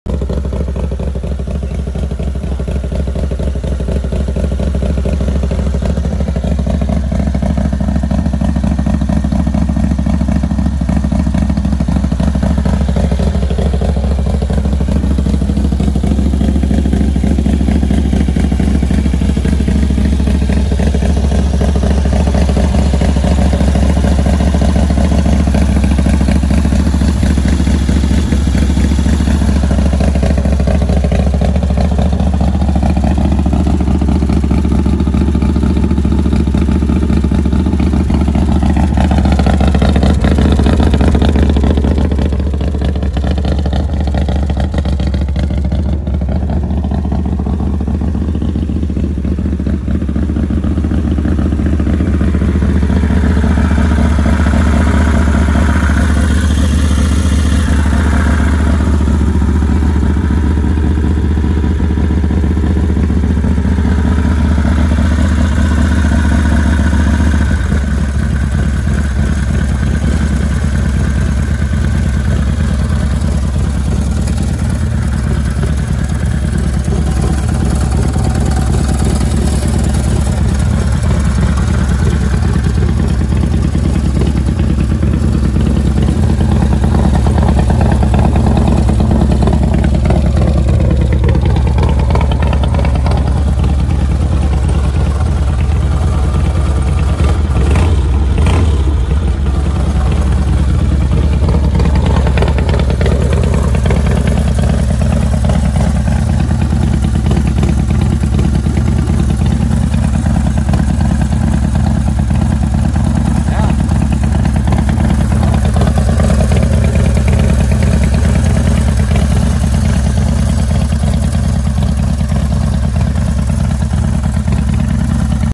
FXR Harley Club Style Ohlins Inverted Front End Fxr Harley Davidson Twin Cam Rebuild 110 Cubic Inch
insta the Owner jduffy39 for questions. Bike is for Sale

110 Cubic inch Twin Cam
Max Power 120 / Max torque 128

Displacement: 1337.00 ccm (81.58 cubic inches)
Engine type: V2, four-stroke
Power: 49.00 HP (35.8 kW)) @ 5000 RPM
Compression: 8.5:1
Bore x stroke: 88.8 x 108.0 mm (3.5 x 4.3 inches)
Valves per cylinder: 2
Fuel control: Overhead Valves (OHV)
Cooling system: Air
Gearbox: 5-speed
Transmission type,
final drive: Belt
Chassis, suspension, brakes and wheels
Front tyre: MM 90-19
Rear tyre: MT 90-16
Front brakes: Single disc
Rear brakes: Single disc
Physical measures and capacities
Weight incl. oil, gas, etc: 276.0 kg (608.5 pounds)
Fuel capacity: 16.00 litres (4.23 gallons)